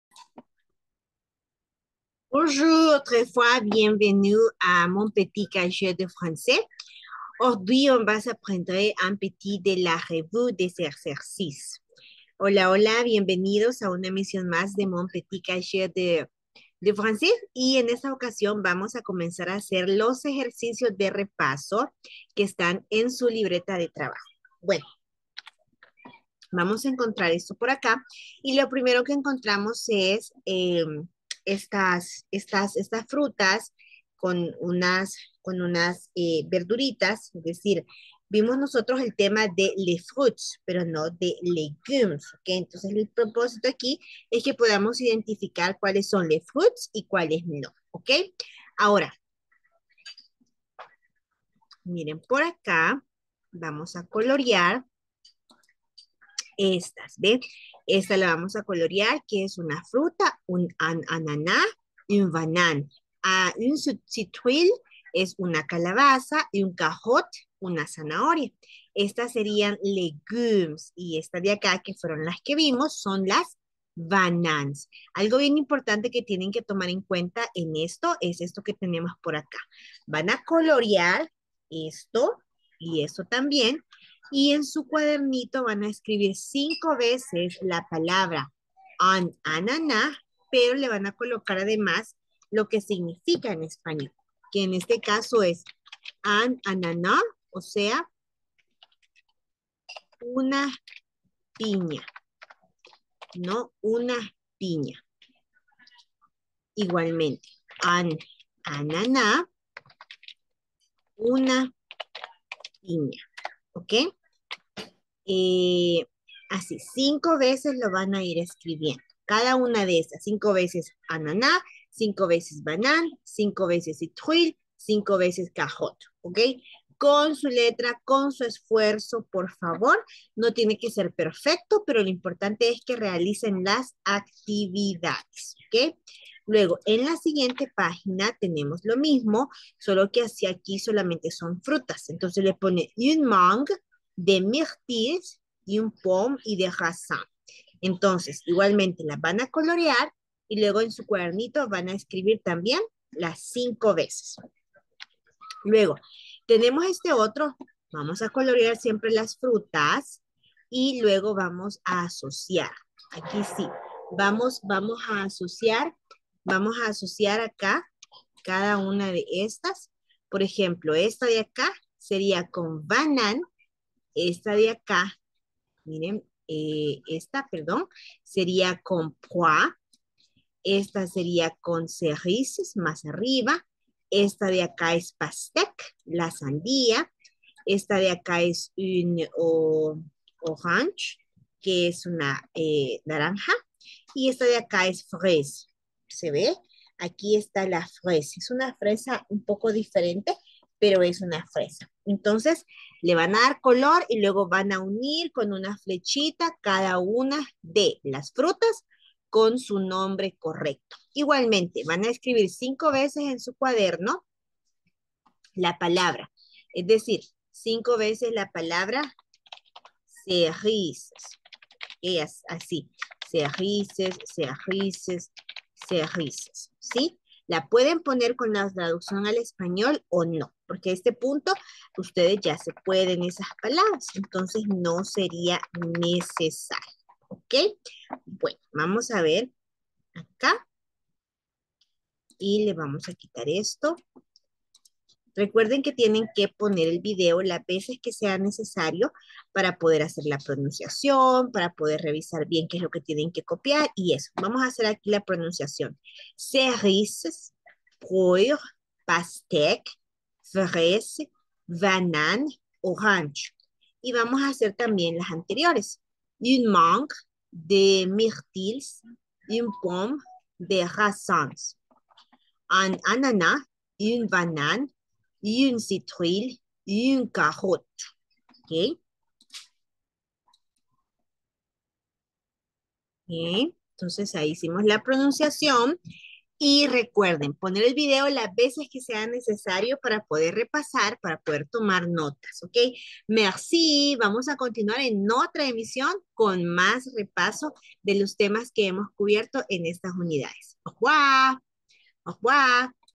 LA REVUE : LES FRUITS
Mon petit cahier de Français  -ESC EDUCACION . Madame  Marisol  Alvarez
Pour enfants de 4 á 8 ans